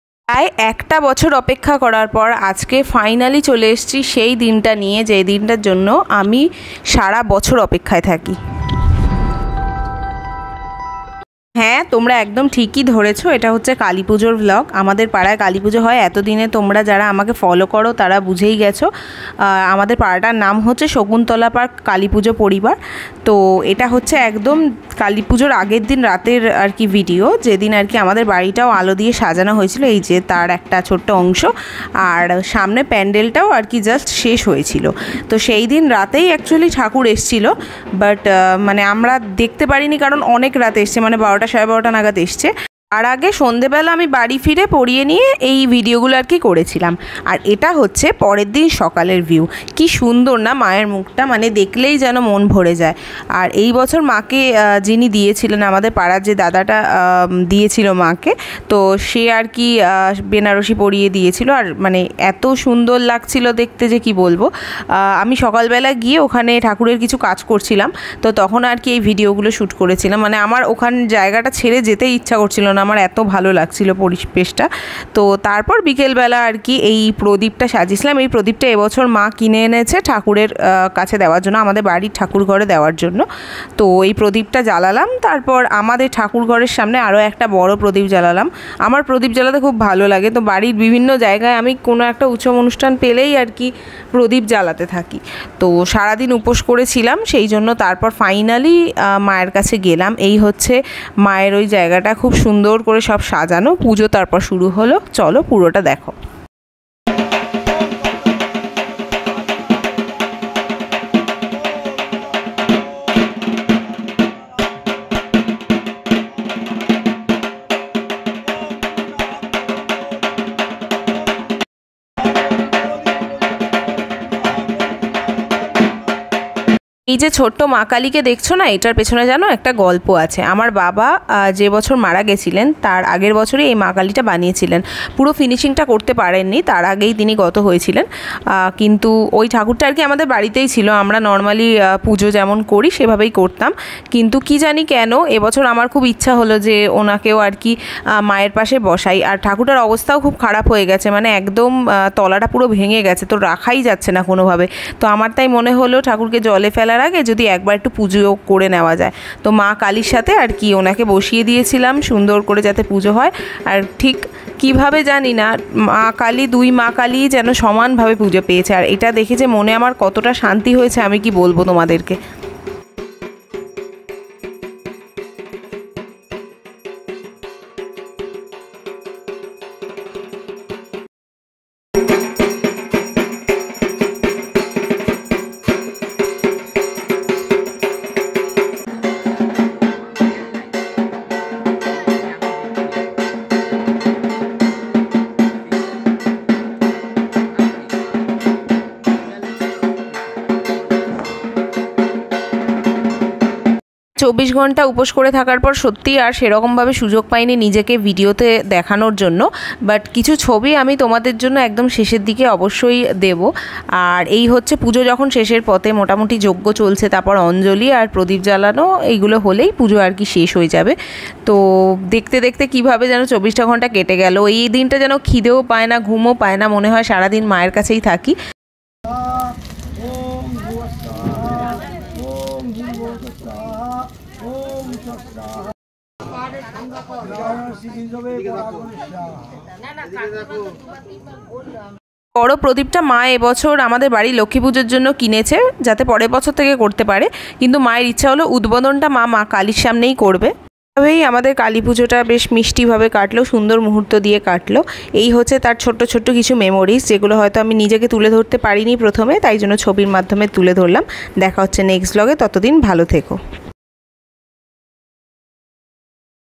২০২৪-এর কালীপূজার প্রথমদিন উদযাপন।
Hello everyone!
Welcome to "MAANI"

Hope you all are doing great.🥰 This is a vlog shoot to cover the first day Of Kalipuja where we had lots of fun. I just want to capture my all memories of this day.. These days are always best 😍🥰, It was a super fun day with lots of laughter. Had some crazy memories. If you people love good memories along with your family then you must watch till the end, also don't forget to mention in the comments what you would love to see next.

Love and positivity always!